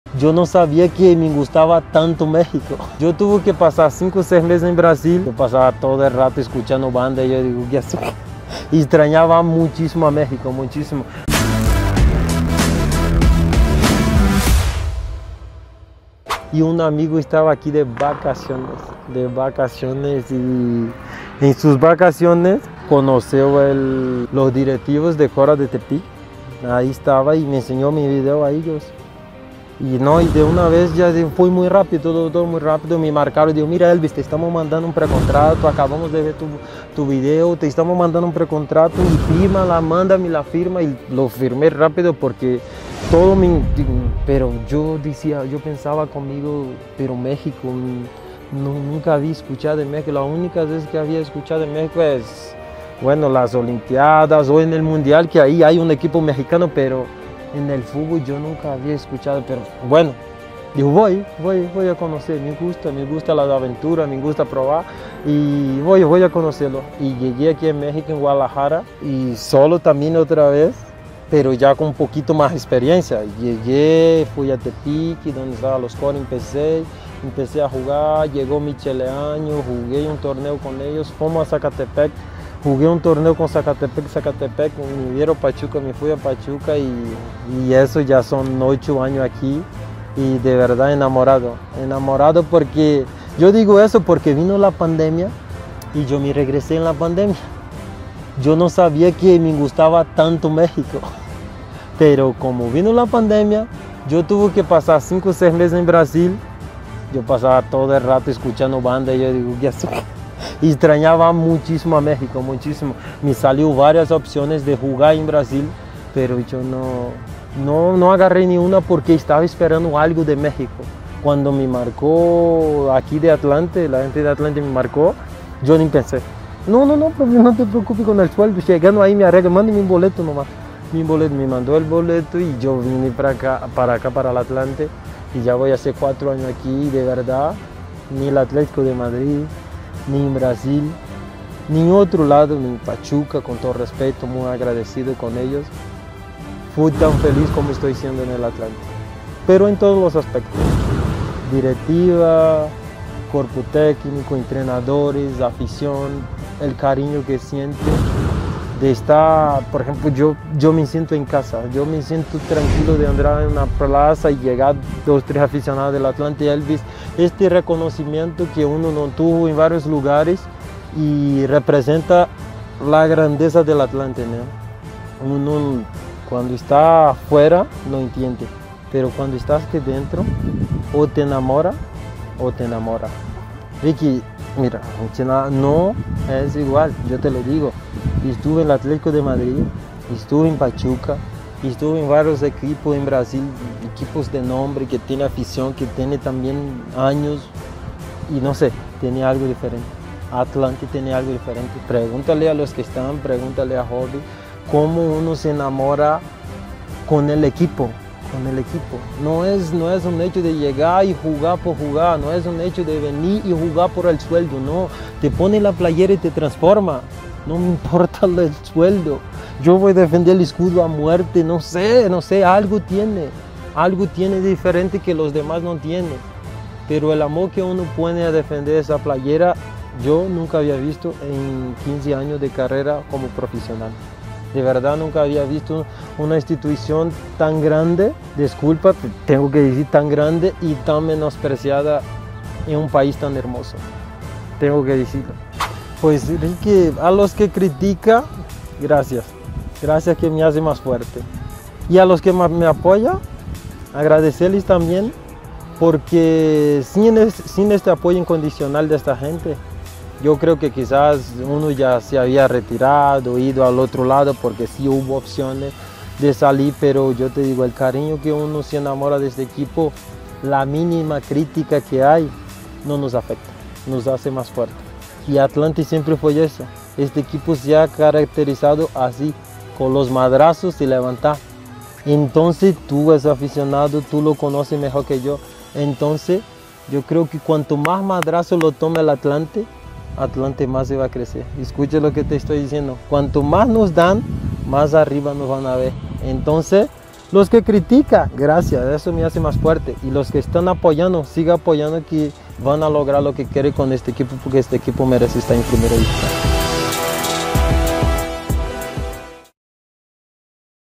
En la 2da parte de la LaCharlaAzulgrana, Elbis Sousa nos habla de su llegada a México
En esta segunda parte de la LaCharlaAzulgrana nuestro PotroDeHierro 🐎 Elbis Sousa nos habla de su llegada a México, su amor por el país y nuestro Club.